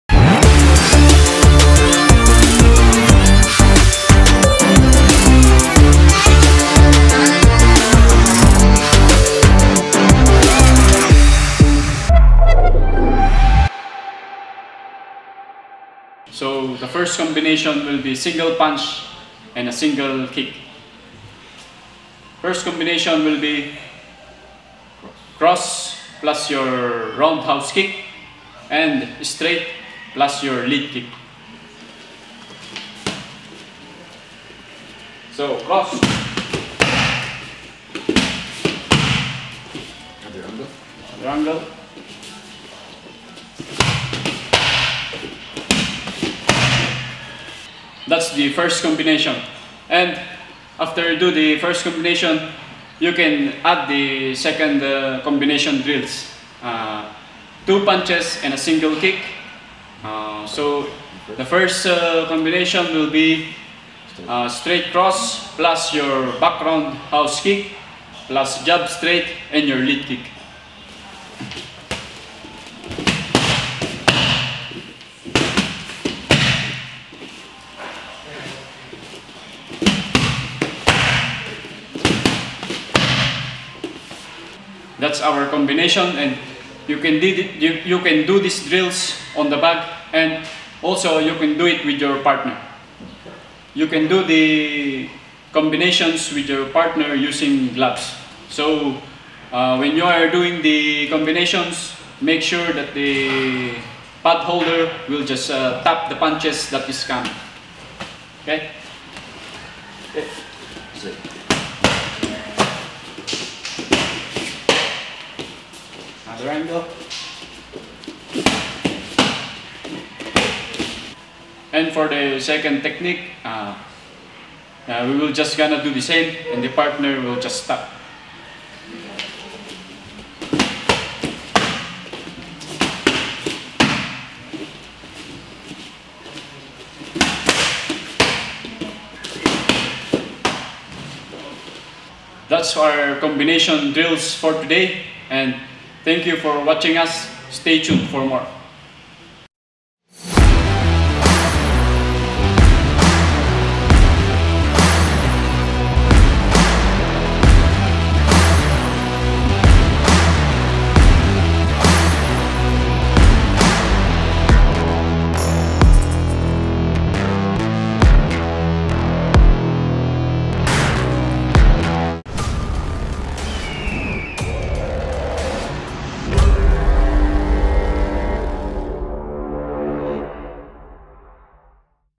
Combination Moves with Stephen "The Sniper" Loman | Team Lakay Instructional
In this video, Stephen "The Sniper" Loman will be teaching us a set of Combination moves that can be used in the cage / octagon.